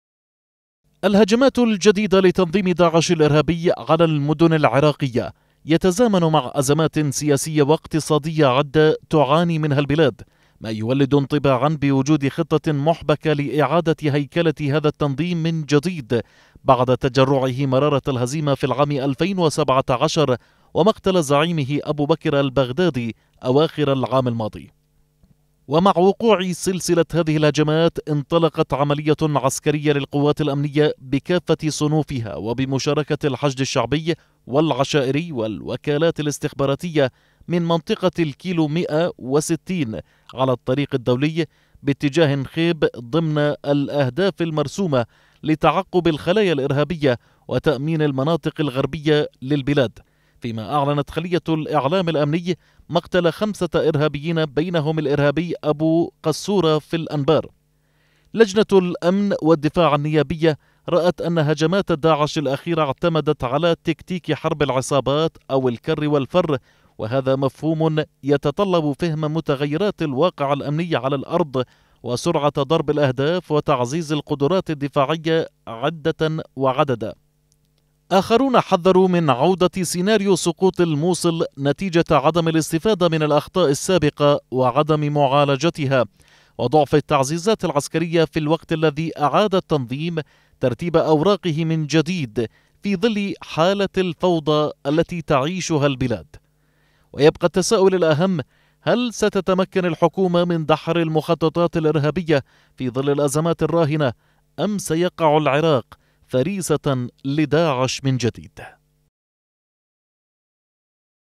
داعش يحاول إعادة تنظيم نفسه في العراق | تقرير
شهدت عدد من المدن العراقية في الآونة الأخيرة هجمات متكررة لتنظيم داعش الإرهابي الذي يحاول احياء نشاطه وتواجده في العراق من جديد بالتزامن من الازمات السياسية والاقتصادية التي تمر بها البلاد.
قراءة: عماد القراله